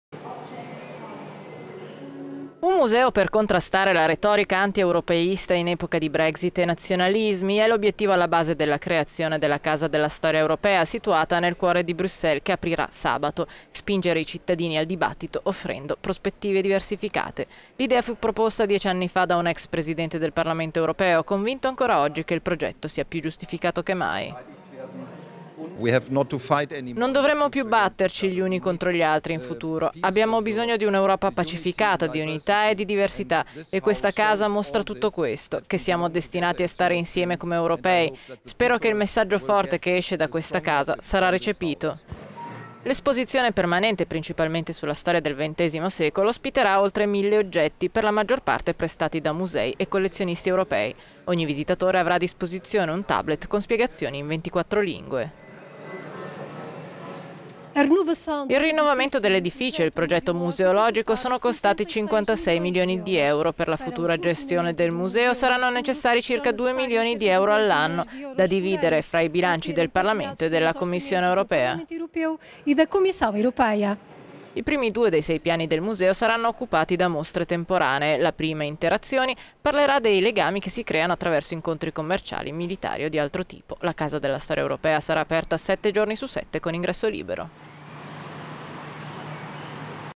Apre a Bruxelles la Casa della storia europea, per contrastare la retorica anti-Ue
Un museo per contrastare la retorica antieuropeista in epoca di Brexit e nazionalismi. È l'obiettivo alla base della creazione della "Casa della storia europea" situata nel cuore di Bruxelles, che aprirà sabato: spingere i cittadini al dibattito offrendo prospettive diversificate.

L'idea fu proposta dieci anni fa da un ex presidente del Parlamento europeo, Hans-Gert Pottering, convinto ancora oggi che il progetto sia…